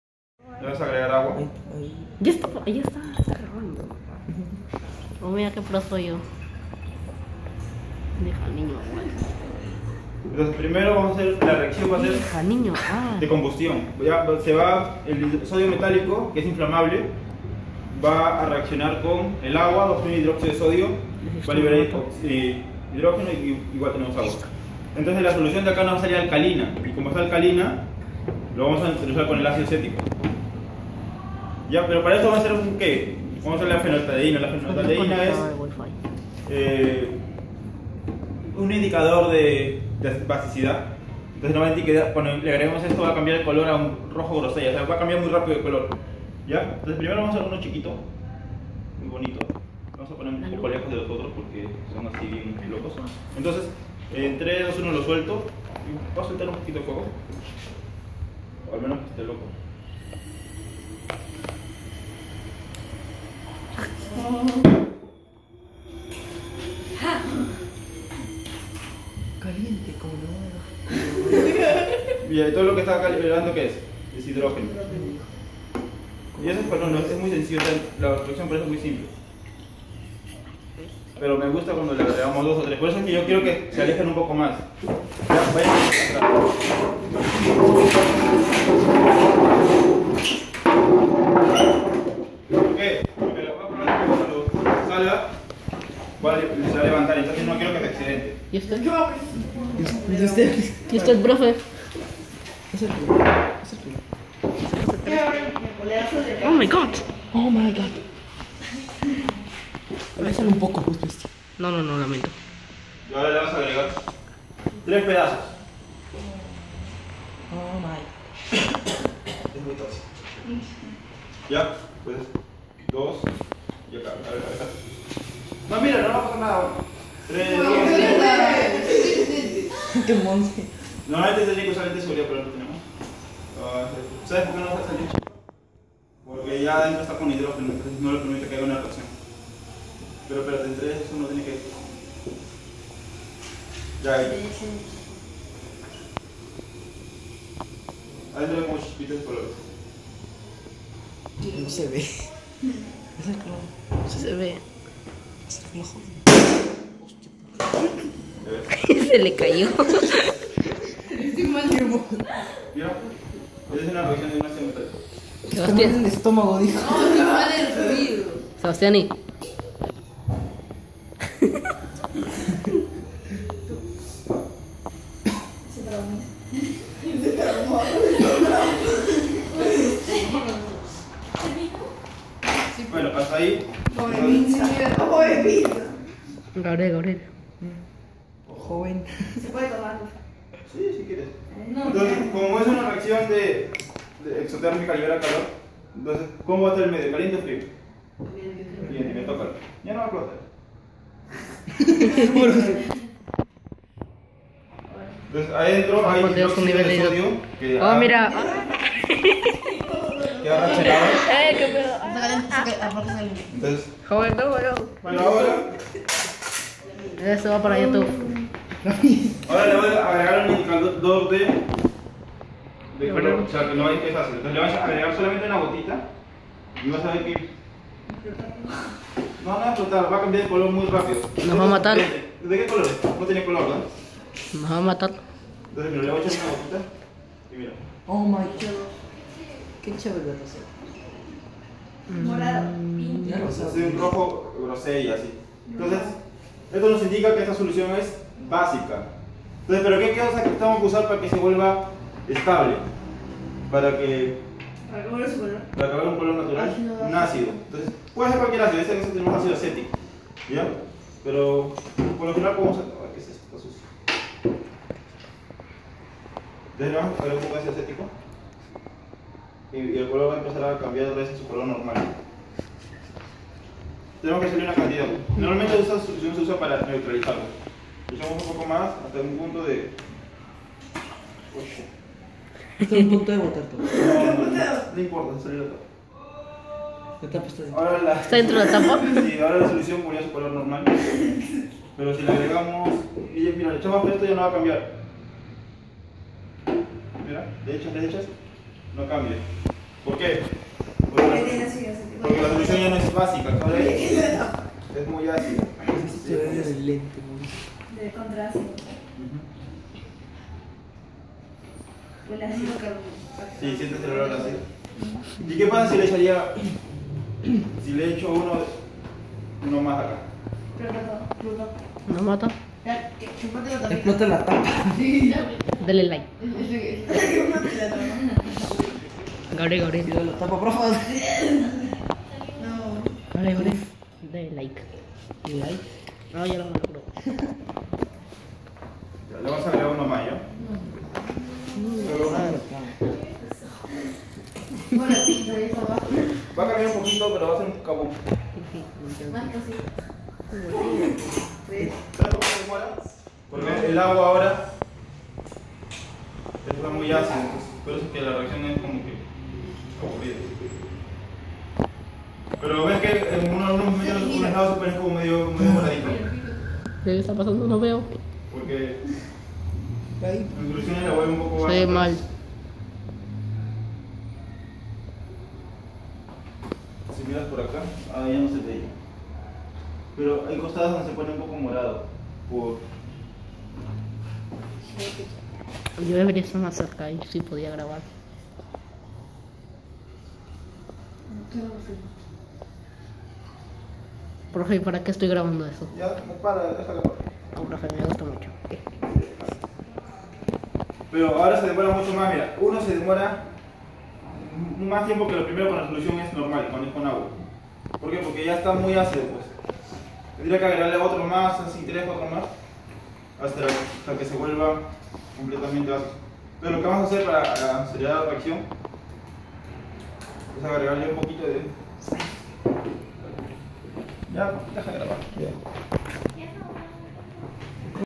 AGUA + SODIO = EXPLOSION
JUGANDO UN POCO CON EL SODIO METALICO Y EL AGUA.
PROVOCANDO UNA REACCION EXOTERMICA Y USANDO LA FENOLTALEINA COMO INDICADOR